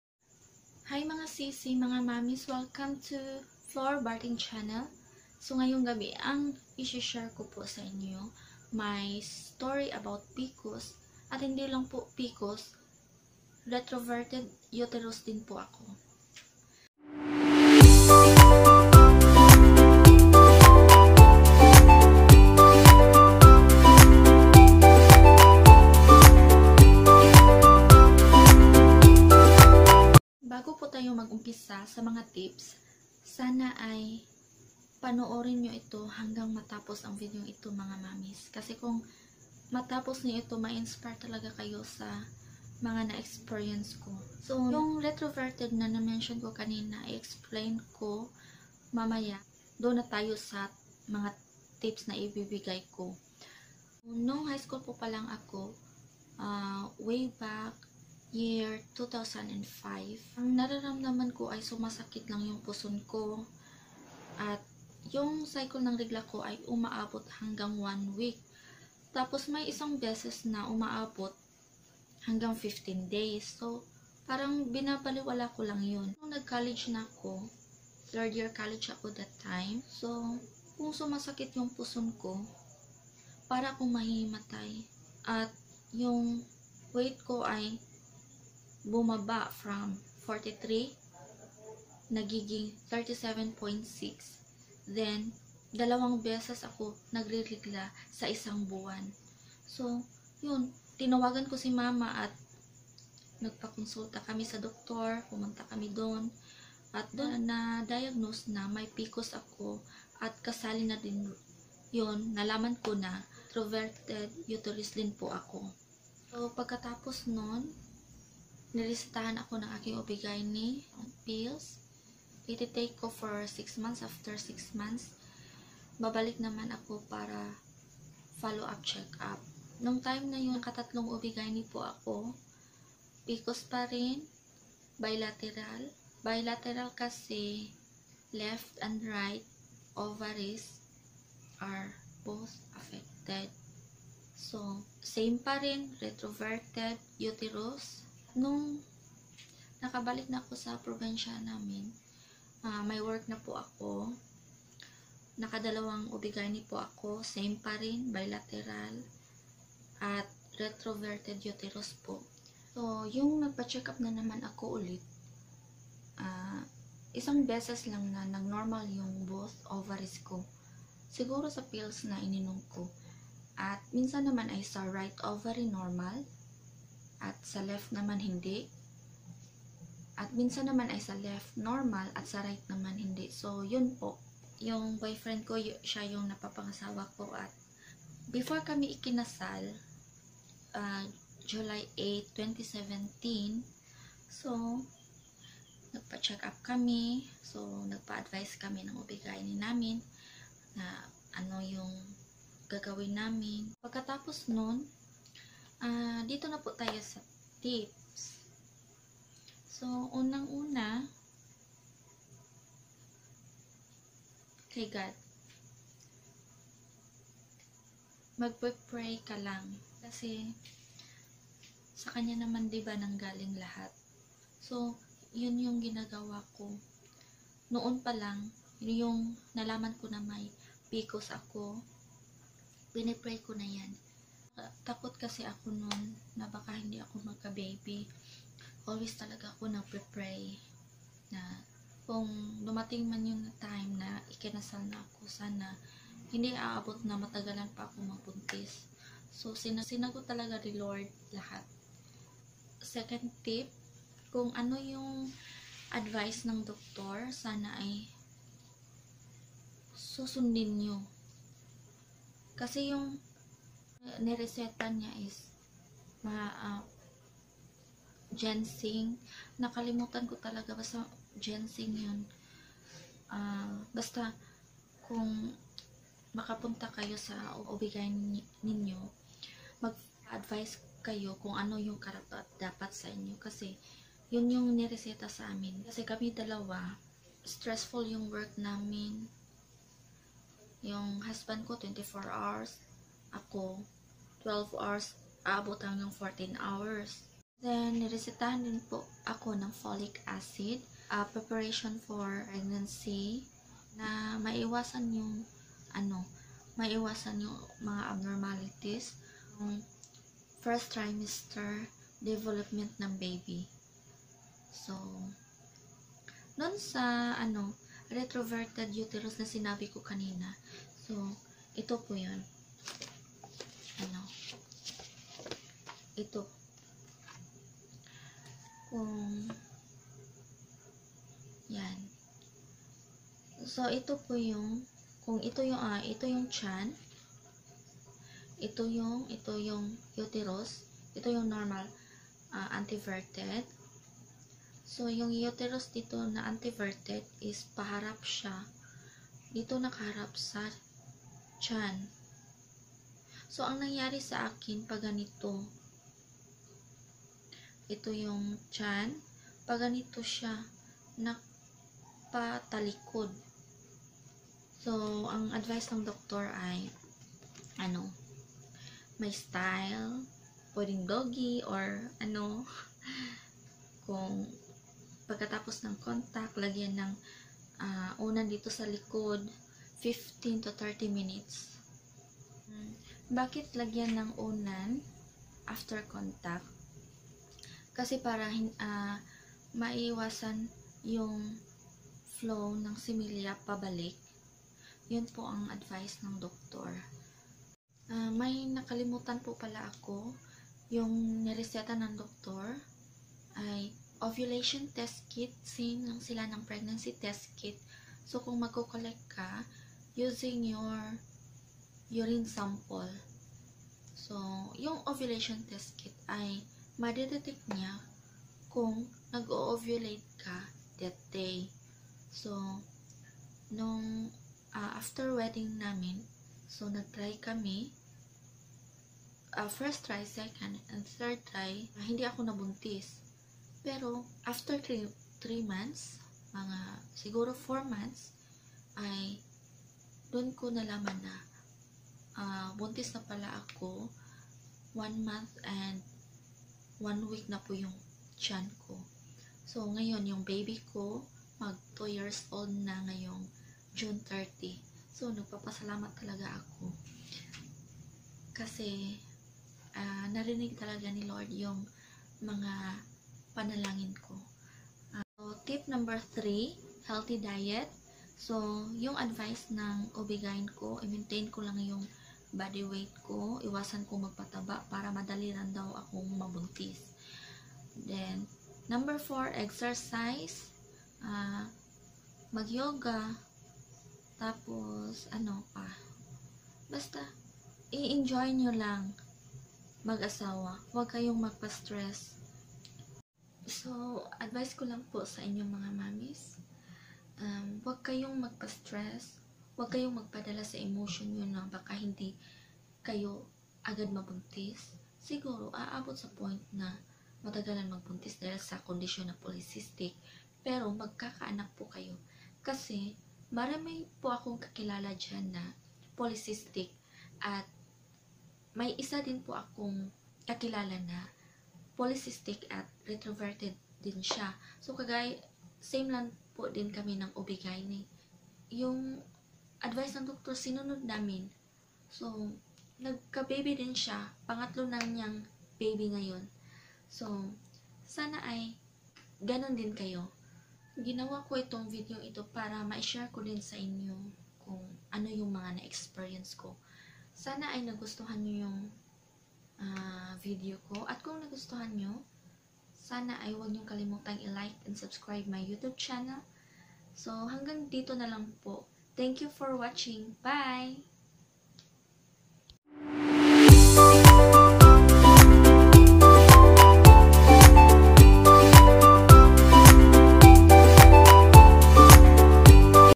My PCOS & Retroverted Uterus Story (Tips) Irregular Menstruation | Paano ako na buntis agad | vlog#1
Hello mga mommies at sa mga mg plano pa lang na mag buntis.
Ginawa ko video ito para ipamahagi sa inyo Kung paano ako na buntis sa condition kung ito.

Sa mga naririnig nyo ay base lamang sa akin naranasan at pag suri ng doctor. Mas mabuti pa rin na mag pa konsulta sa doctor para may tamang gamot pa ra sa inyo.

Sana ay nag enjoy kayo sa 1st vlog ko at Kung na gustohan nyo.